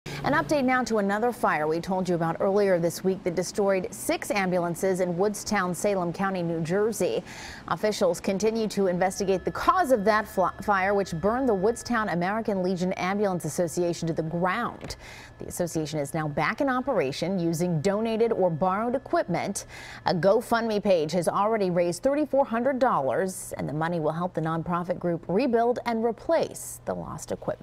Fire-Ravaged Ambulance Company Back In Service
Officials continue to investigate the cause of the blaze, which burned the Woodstown American Legion Ambulance Association to the ground.